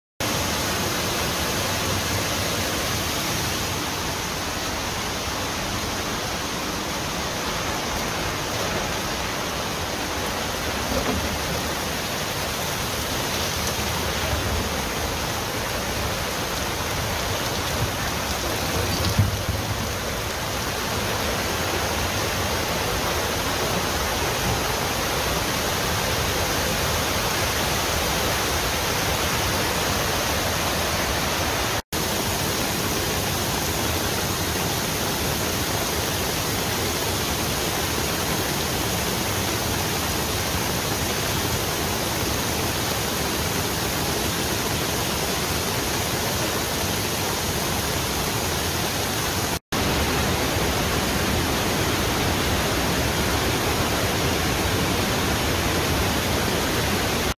'Who Needs Experiments?': Water Researcher Sees Theory Become Reality as Manly Dam Spills
Credit: Mitchell Harley via Storyful